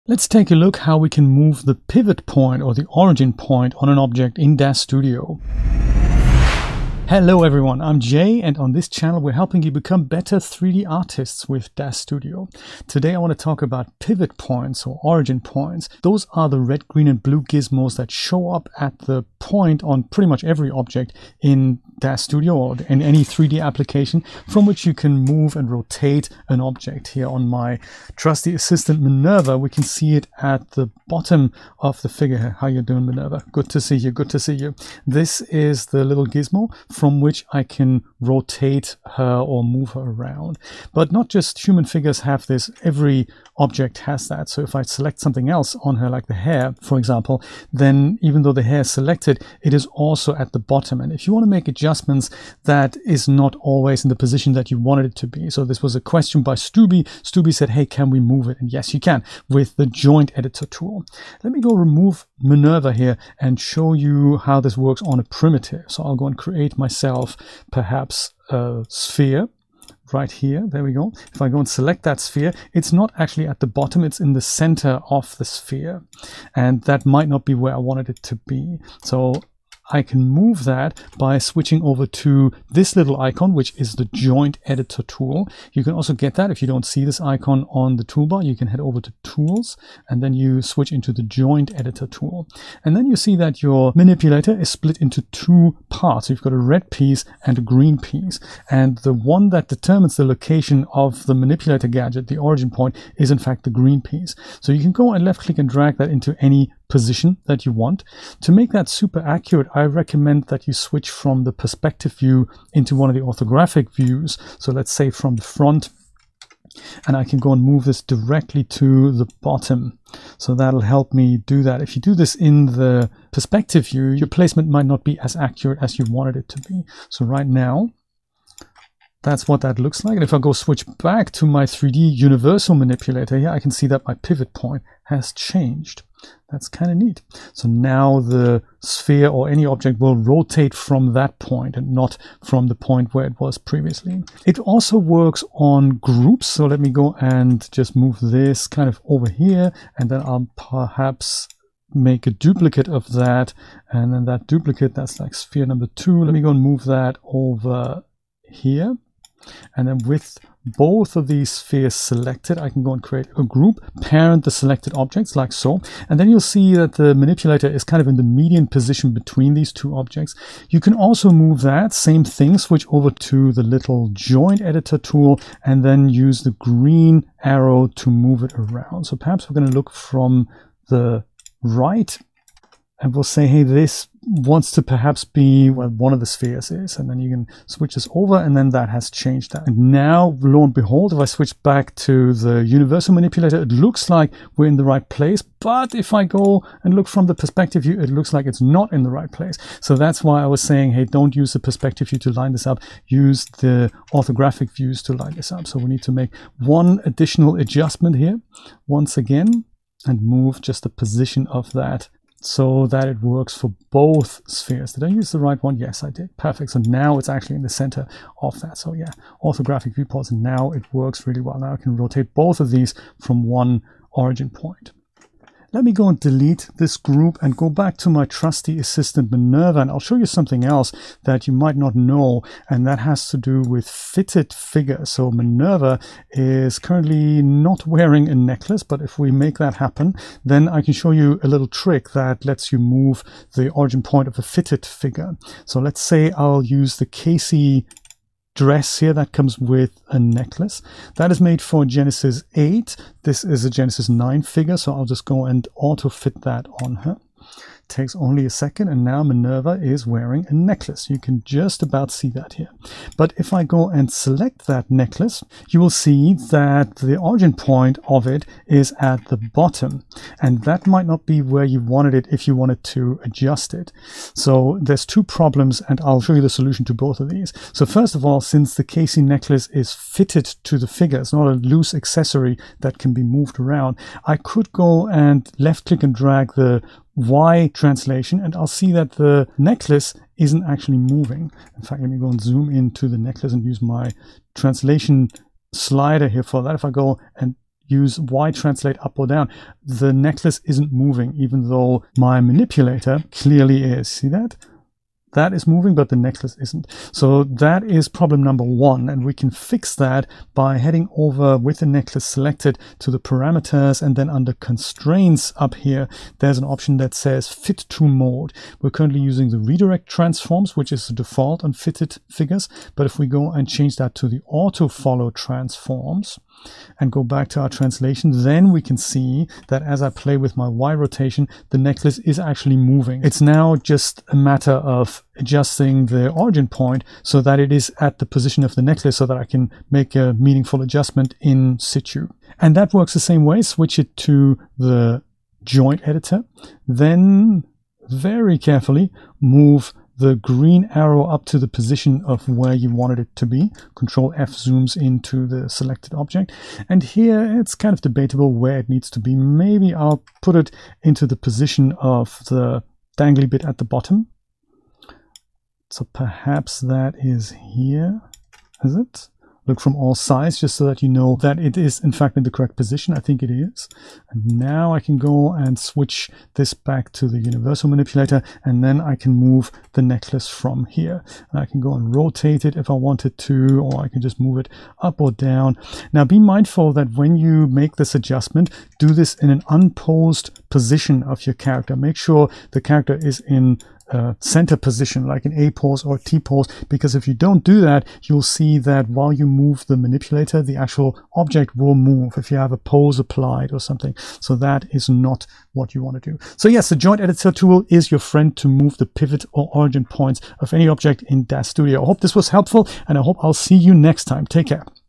Adjusting Pivot Points on Objects in Daz Studio
In this episode, Jay shows you how to adjust pivot points (or origin points) in Daz Studio. It's the spot around which objects rotate, and it's not always in the most convenient position. Jay explains how to move it on props as well as fitted items in your scene.